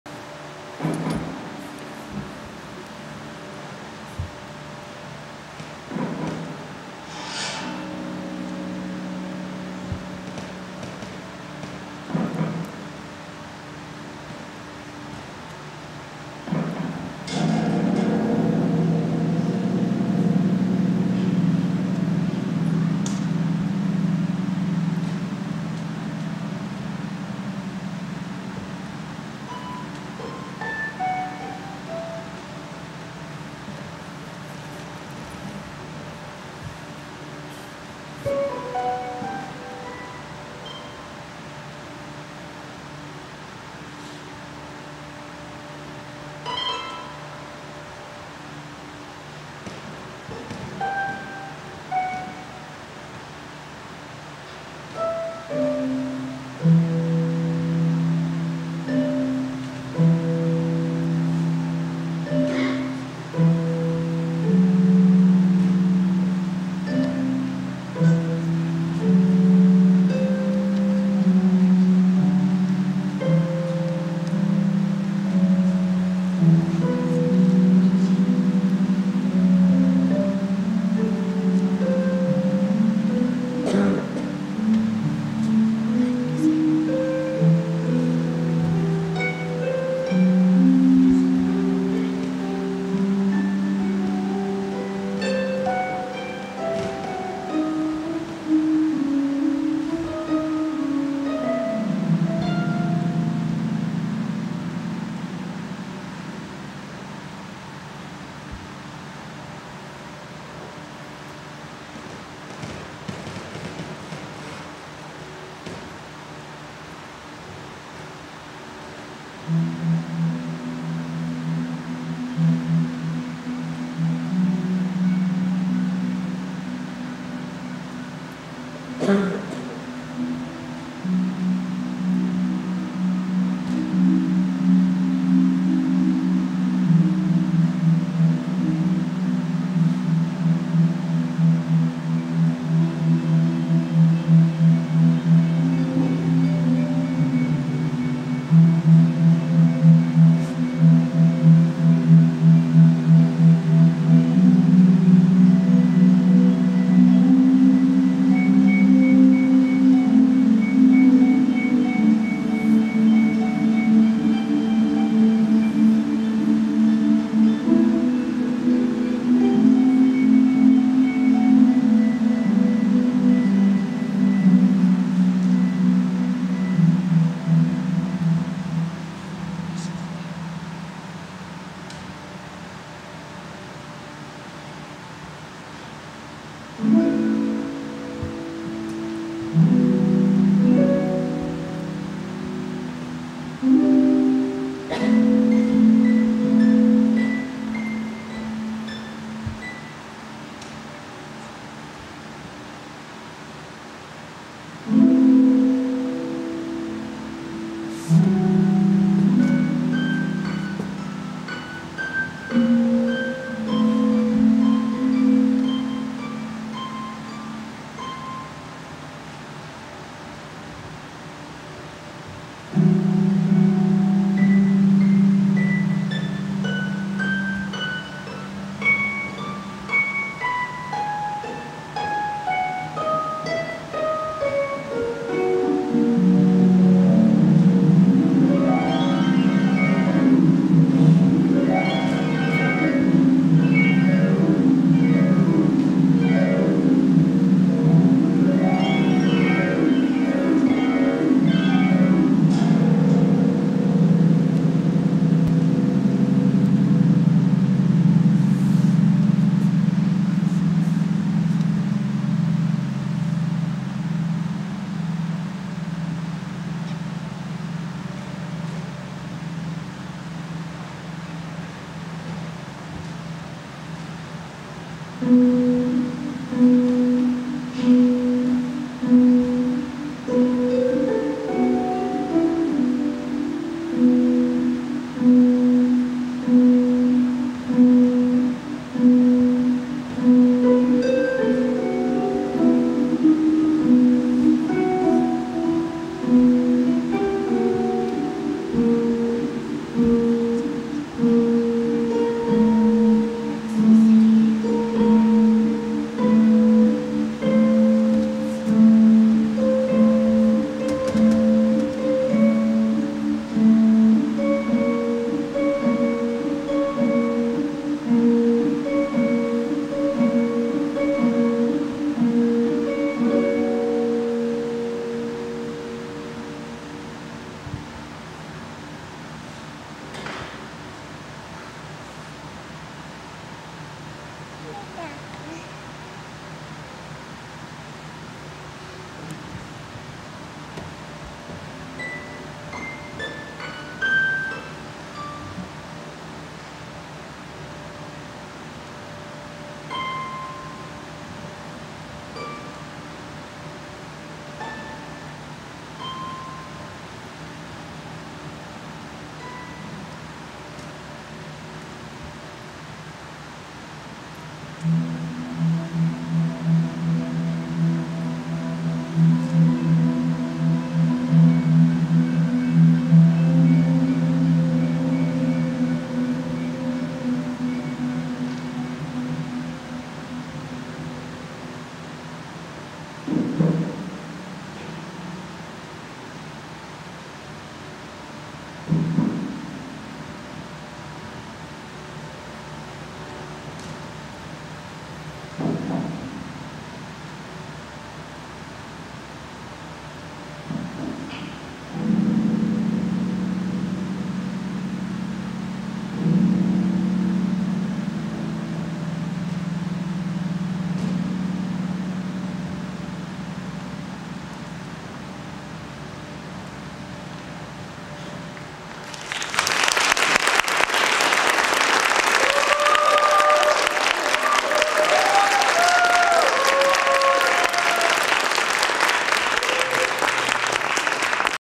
Sketches of Rodin
This version performed by Kalindi Stockton.  Choreography by Sarah Morrison with original composition by James Bonney.  Performed at the MorrisonDance 10th anniversary presentation during Cleveland Public Theatre's DanceWorks series.